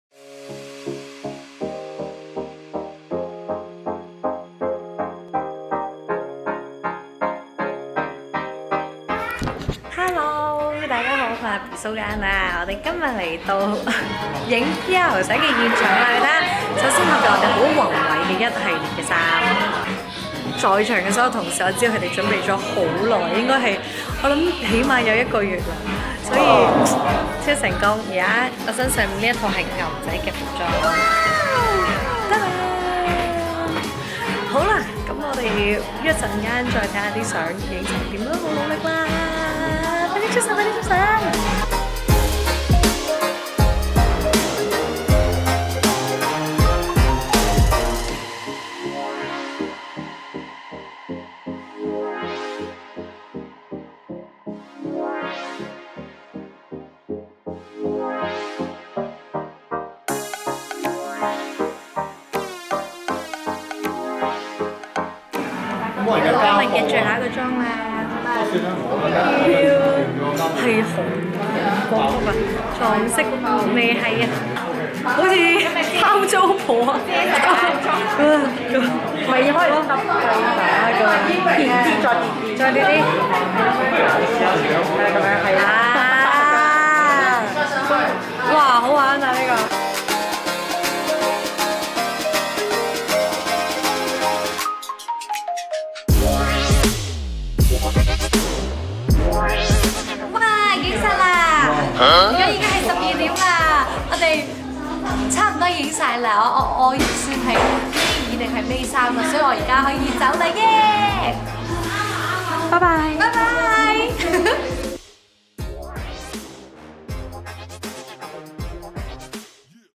【OUR SONG CHAPTER】Behind the Camera//蘇家欣篇
2021 Song Entertainment 尚品娛樂// Reassemble
哇哇哇，係籃球女神啊Bee蘇家欣同大家睇幕後啊！
睇到啊Bee今次咁多個造型，你覺得邊個最啱佢呢？
靜靜地同你講，期待下一集啦！

Photographer : Leozting
Make Up and Hair :  Sarah@WingKee Make Up, Ray Hui
Stylist : Ceci Production